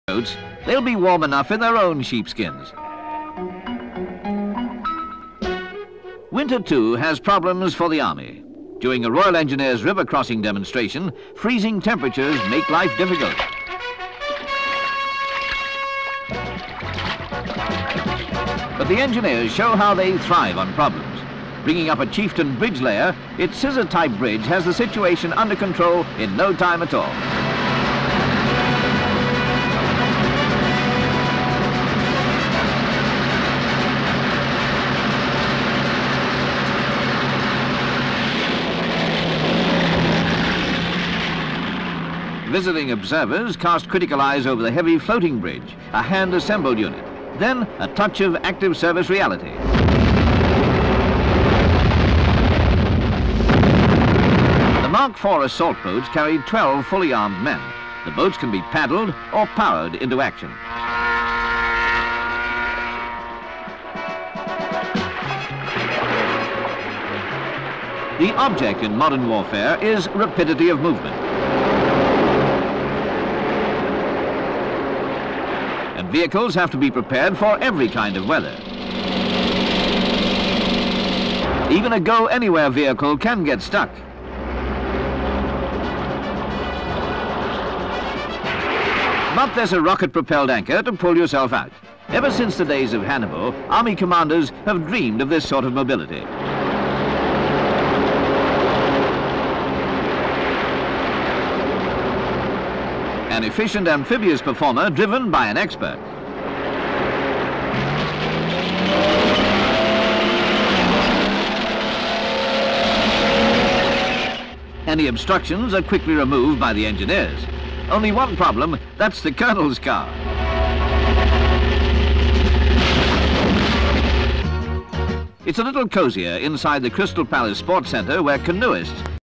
RIVER CROSSING DEMONSTRATION - (Frozen River Scenes) - COLOUR
(29 Feb 1976) The Royal Engineers give a demonstration of how the modern Army bridges a river.